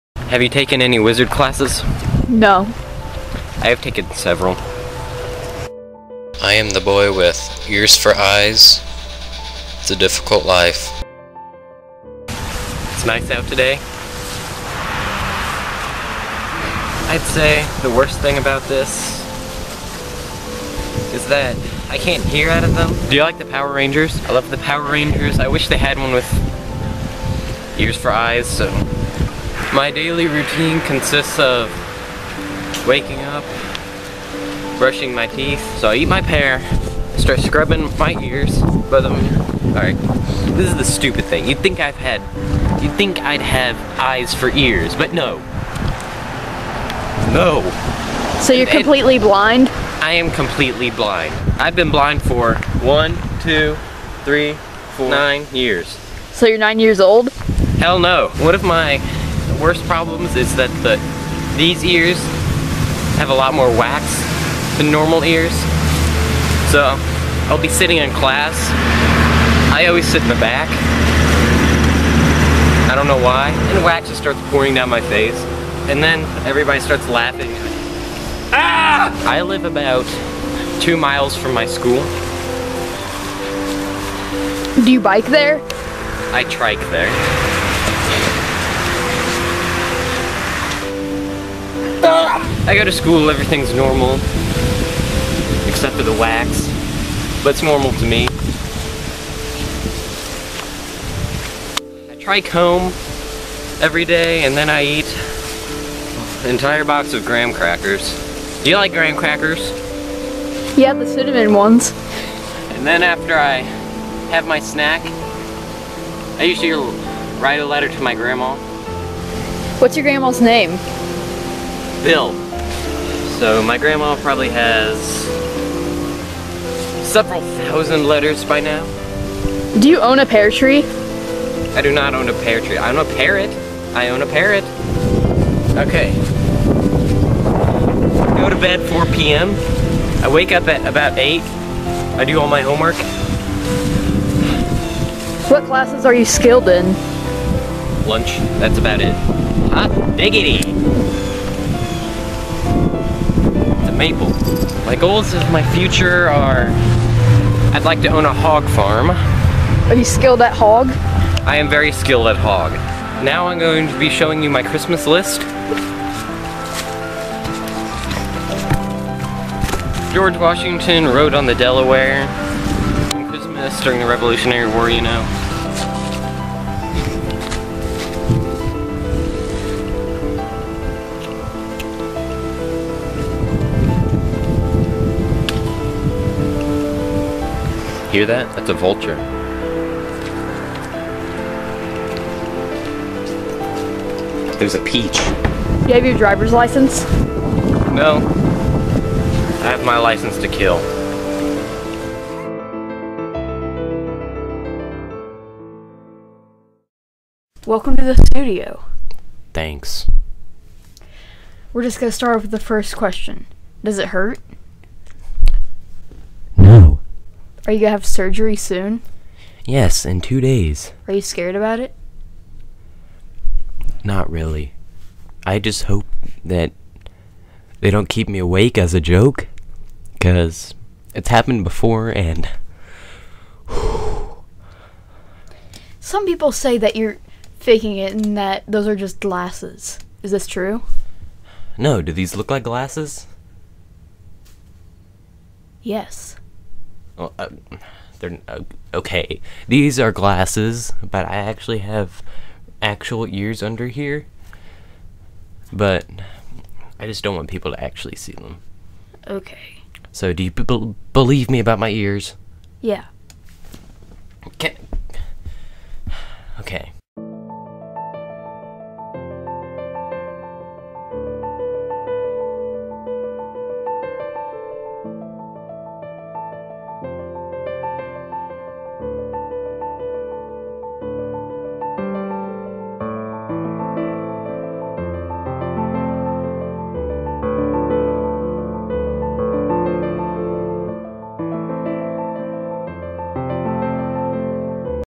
Born With Ears For Eyes
-We go inside the life of a poor boy born with ears for eyes.

For more videos like these, go to my channel!